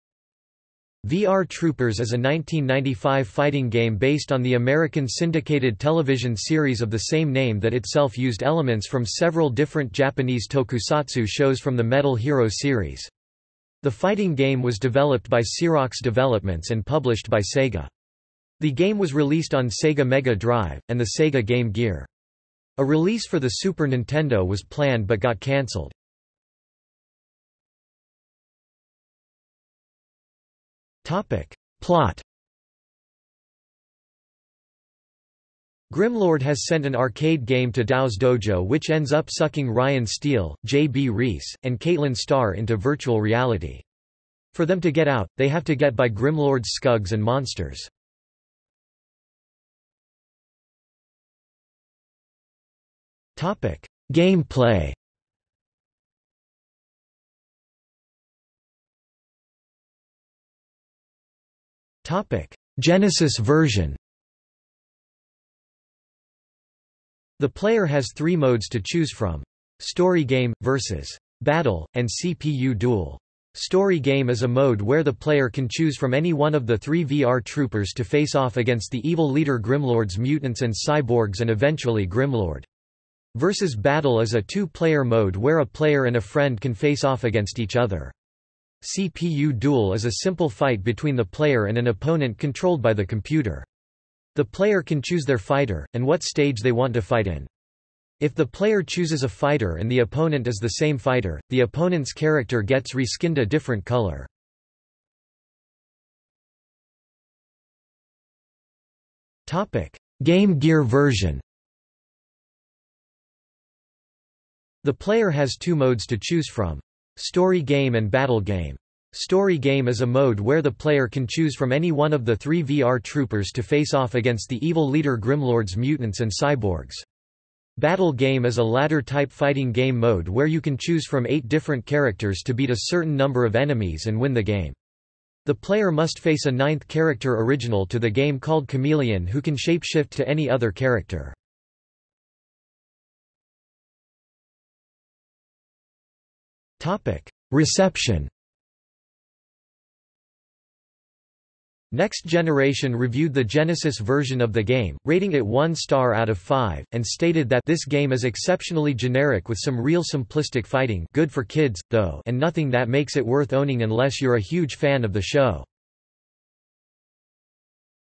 VR Troopers (video game) | Wikipedia audio article
00:00:27 1 Plot
00:00:49 2 Game play
00:00:58 2.1 Genesis Version
00:01:46 2.2 Game Gear Version
00:02:23 3 Reception

- Socrates

SUMMARY
VR Troopers is a 1995 fighting game based on the American syndicated television series of the same name that itself used elements from several different Japanese tokusatsu shows from the Metal Hero Series. The fighting game was developed by Syrox Developments and published by Sega. The game was released on Sega Mega Drive, and the Sega Game Gear. A release for the Super Nintendo was planned but got cancelled.